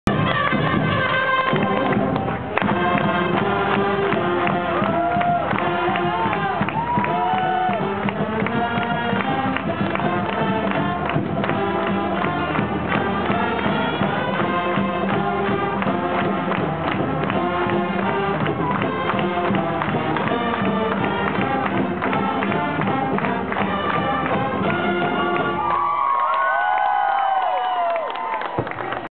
Cheerleaders and dance team
This is the dance to the fight song performed by our cheerleaders and dance team